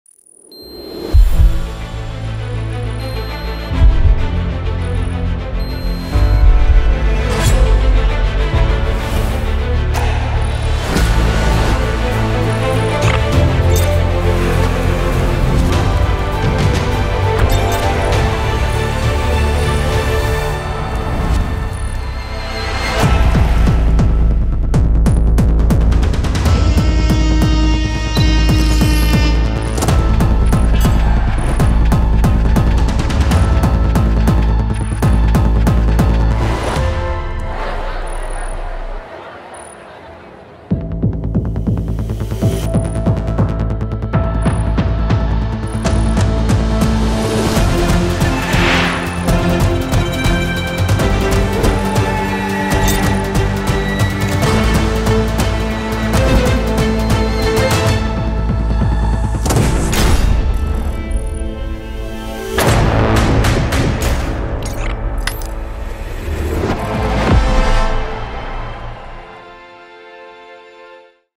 Money2020 Event Trailer for Europe 2022
The event you and your business can't afford to miss! Money2020 EU 2022 is coming in less than 2 weeks...